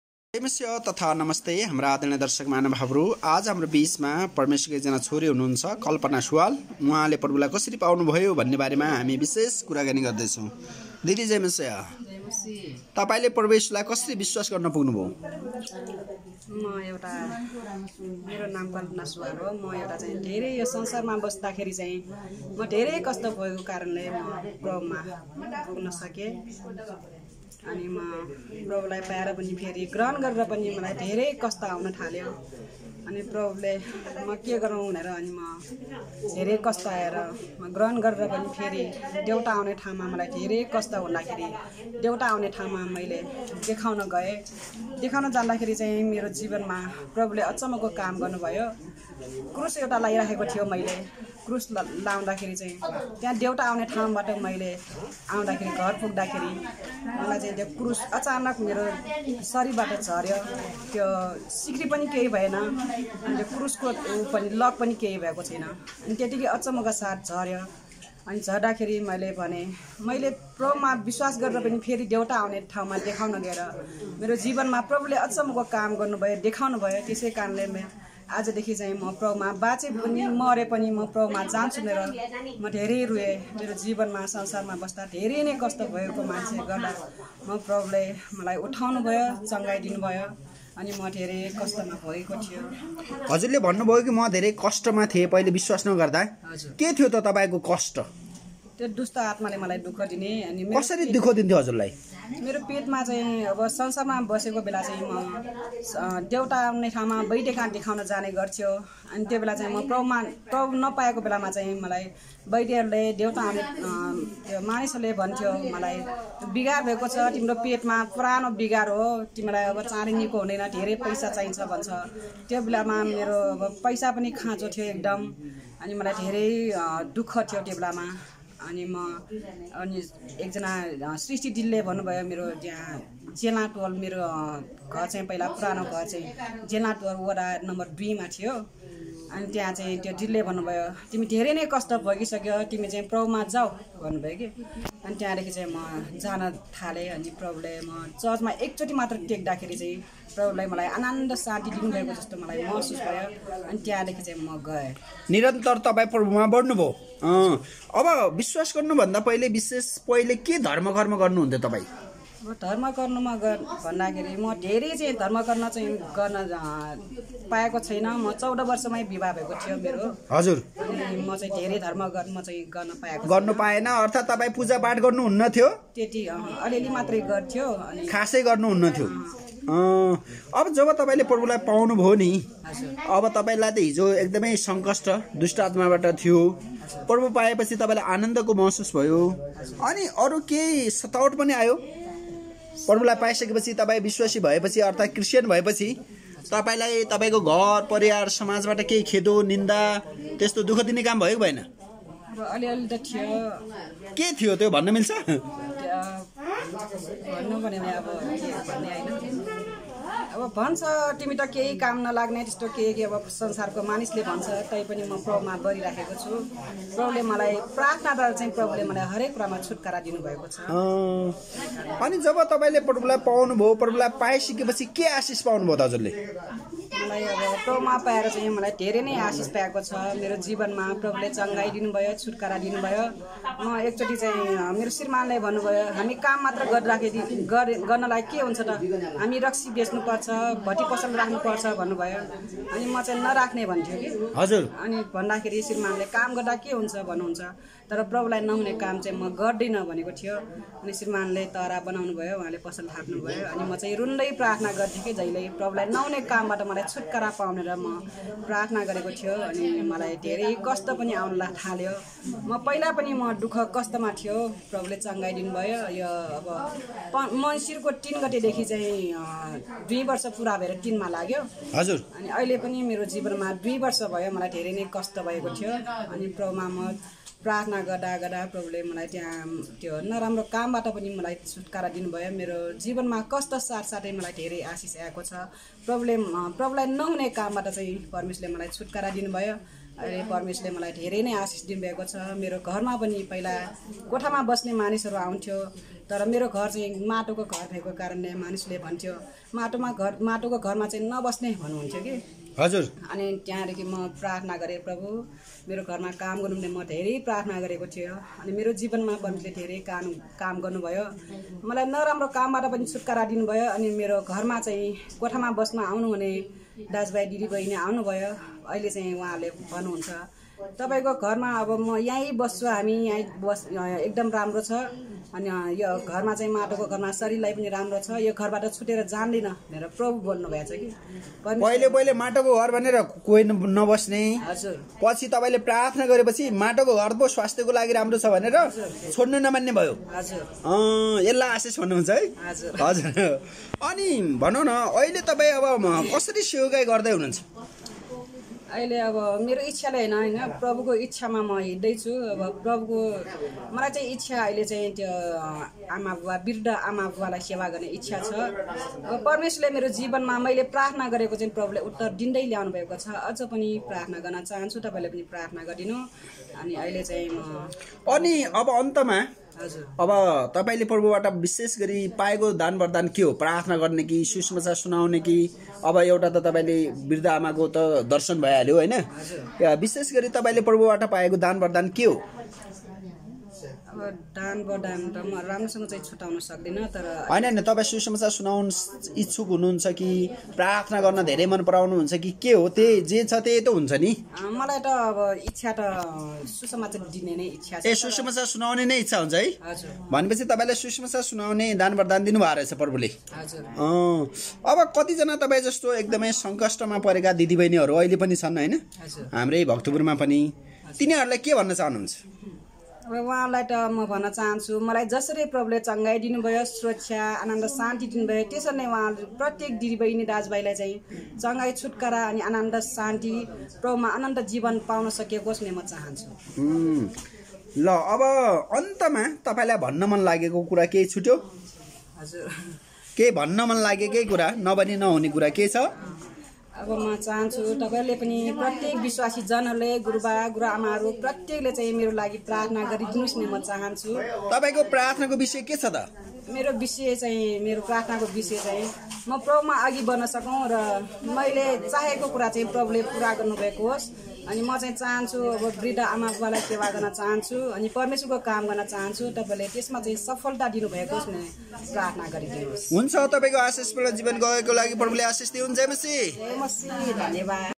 १४ बर्ष दुष्टले दुख पिडा ,रोदन छ्टपटी ,कष्ट आँसु सबै दियो।।।KALPANA SUWAL
LIFE TESTIMONY OF KALPANA SUWAL
JAYMASHIH TV